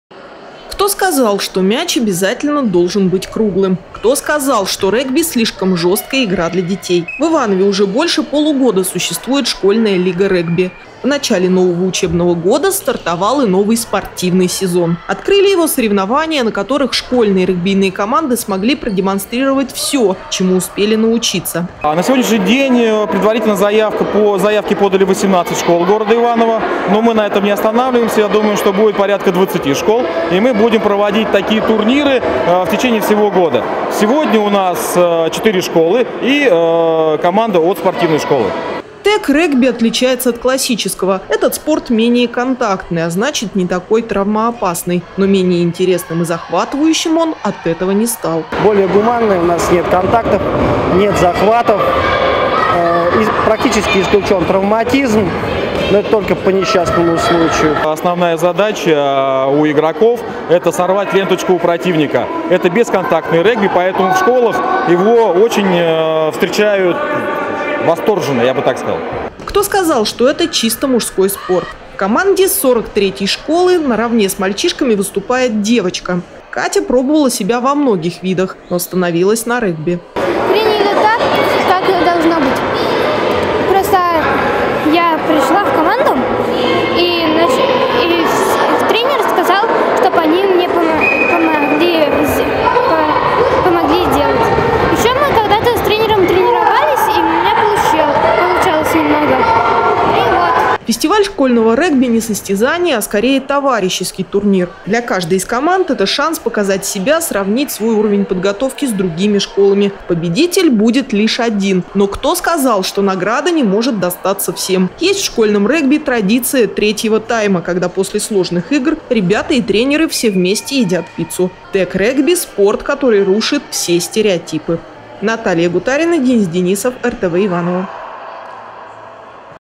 Тэг-регби против спортивных стереотипов
Кто сказал, что регби – слишком жесткая игра для детей? В Иванове уже больше полугода существует школьная лига регби. В начале нового учебного года, стартовал и новый спортивный сезон. Открыли его соревнования, на которых школьные регбийные команды смогли продемонстрировать все, чему успели научиться.